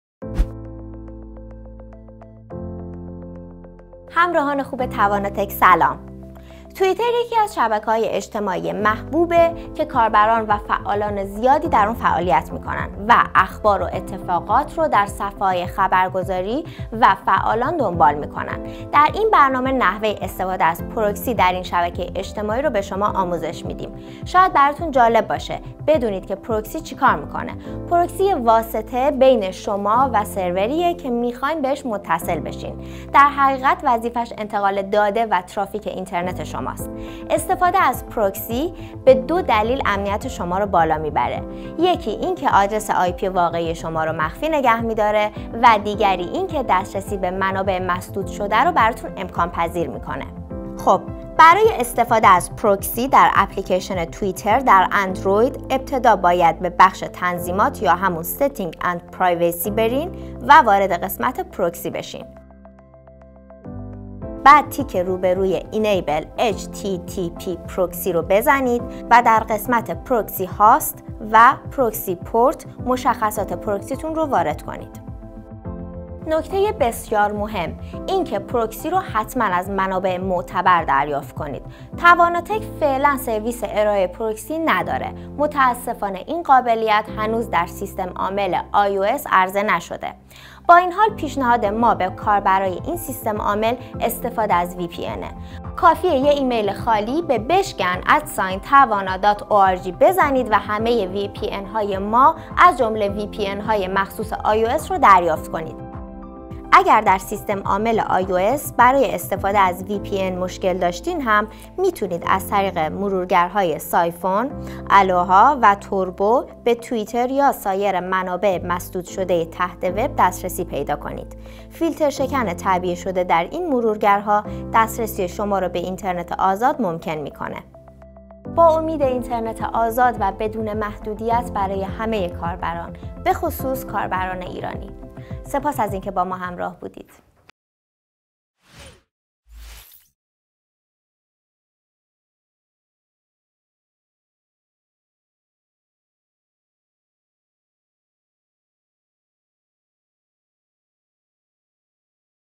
چگونه از پروکسی در توییتر استفاده کنیم؟
توئیتر یکی از شبکه‌های محبوب و پرطرفداراست که کاربران و فعالان زیادی در آن فعالیت می‌کنند.
در این برنامه با نحوه استفاده از پروکسی برای توئیتر آشنا می‌شویم.